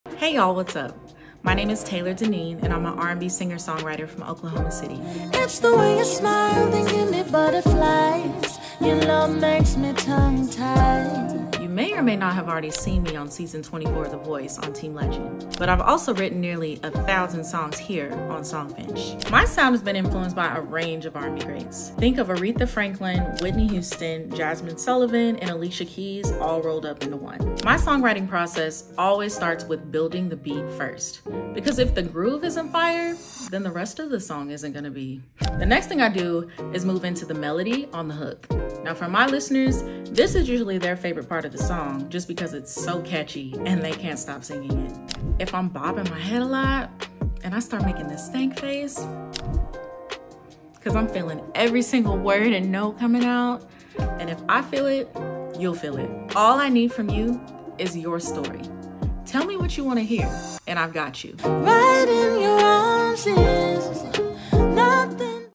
Meet Songfinch Artist: Taylor Deneen
Meet Taylor Deneen, the Oklahoma City-born singer/songwriter with influences spanning R&B, Soul, Pop, and Jazz. With a Berklee College of Music education (2021) and hundreds of custom songs crafted on Songfinch, she's known for infusing every note with heart and soul, delivering a truly personalized musical experience. Taylor just finished up an impressive run on Season 24 of NBC's The Voice, where she made it to the Top 20 and was acclaimed the “best soul singer” of blind auditions by John Legend himself. Taylor Deneen promises to transform your stories into unforgettable melodies.

Learn more about Taylor Deneen & work with her to create your custom song